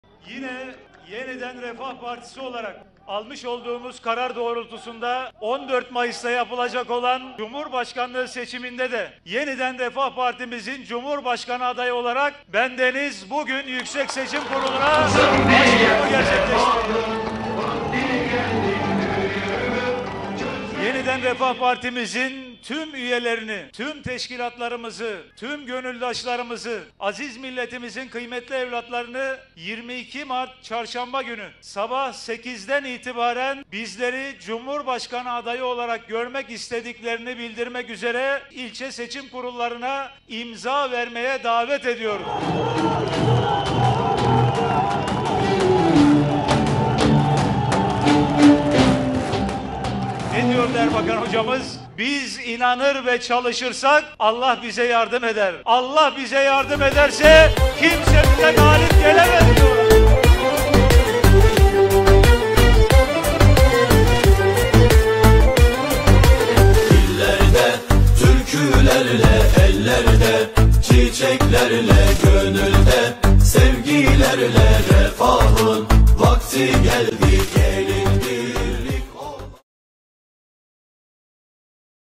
Aziz milletimizi ilçe seçim kurullarına imza vermeye davet ediyoruz.
Tüm teşkilatlarımızı, üyelerimizi, gönüldaşlarımızı ve aziz milletimizi 22 Mart Çarşamba günü sabah 08.00’den itibaren, bizleri Cumhurbaşkanı adayı olarak görmek istediklerini belirtmek üzere ilçe seçim kurullarına imza vermeye davet ediyoruz.